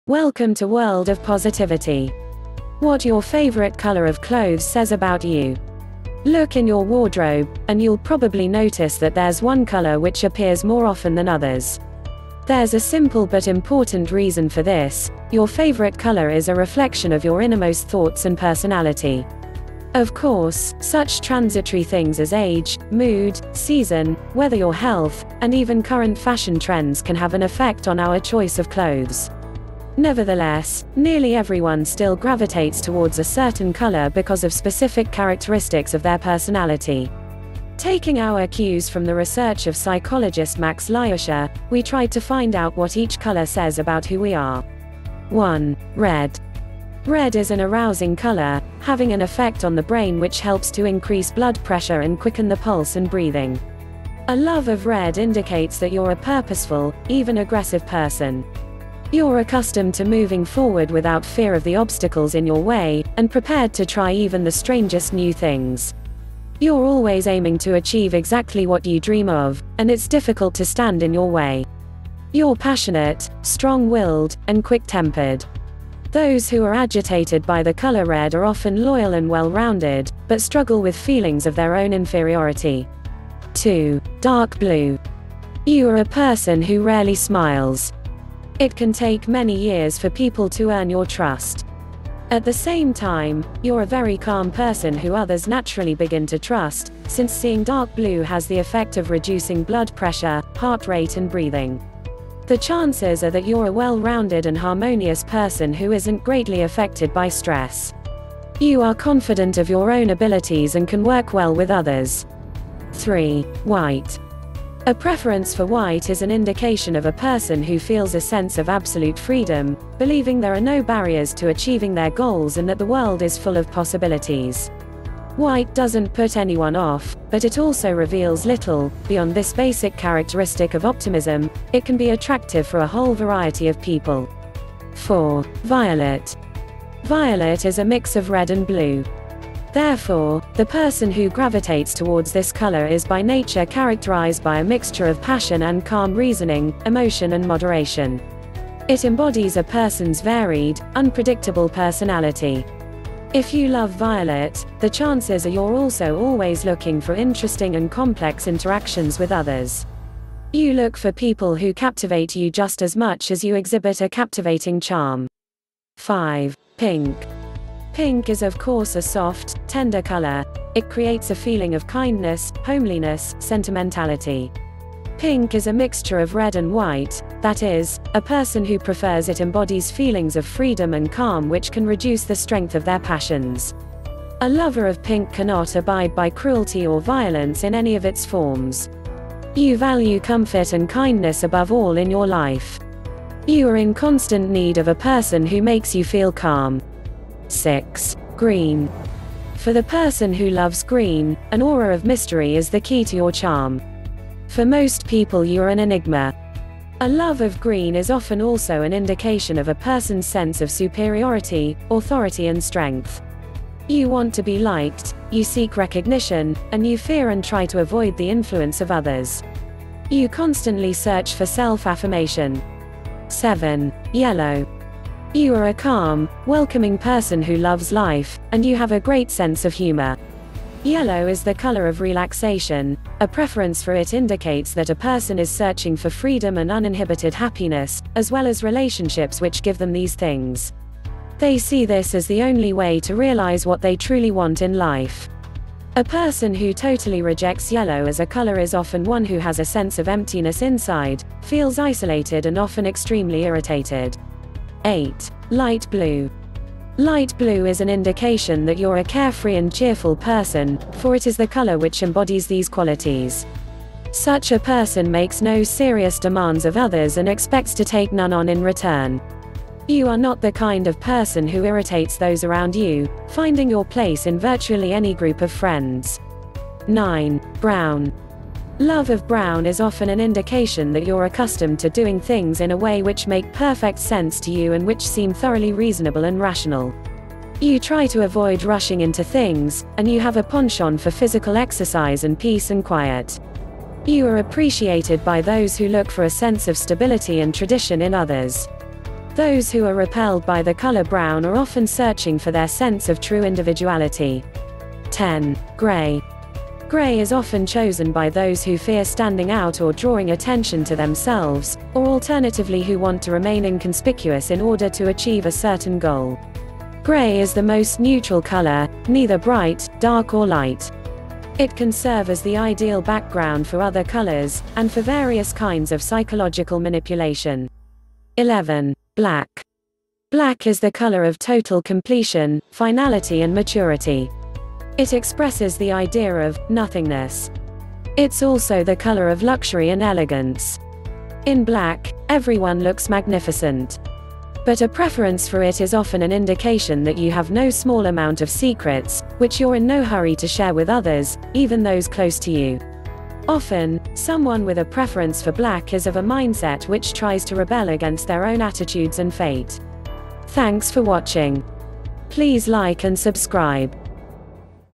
WHAT YOUR FAVOURITE COLOUR OF CLOTHES SAYS ABOUT YOU
Look in your wardrobe, and you'll probably notice that there's one colour which appears more often than others. There's a simple but important reason for this: your favourite colour is a reflection of your innermost thoughts and personality. Of course, such transitory things as age, mood, season, weather your health, and even current fashion trends can have an affect on our choice of clothes. Nevertheless, nearly everyone still gravitates towards a certain colour because of specific characteristics of their personality.

Taking our cues from the research of psychologist Max Lyusher, we tried to find out what each colour says about who we are.